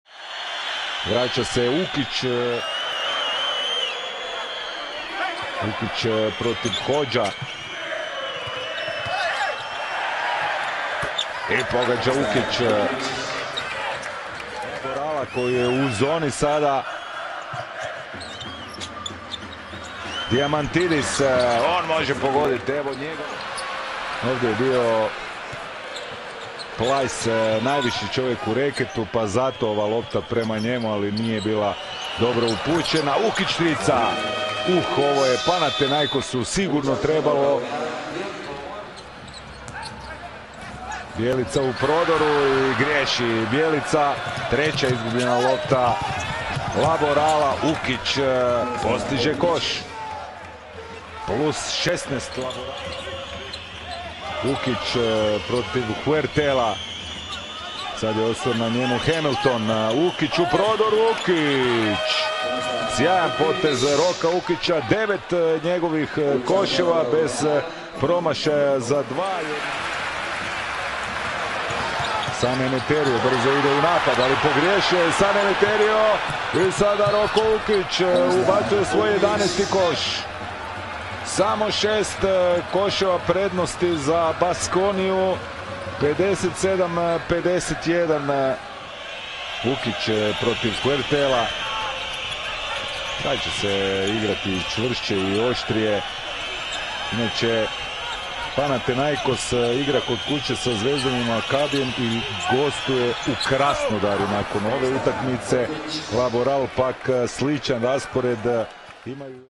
Roko Ukic vs Laboral Kutxa 29/11/13
(video rights reserved by Euroleague)
This Friday (10/01/14) Panathinaikos is away at Laboral Kutxa in Fernando Buesa Arena for the second time this season. PaoBC was unlucky last time around, losing 79:77, and Roko Leni Ukic scored 14 points in that game. If RLU plays like this on Friday, Pao will be close to a big win away from OAKA.
ελα ρε παναθα!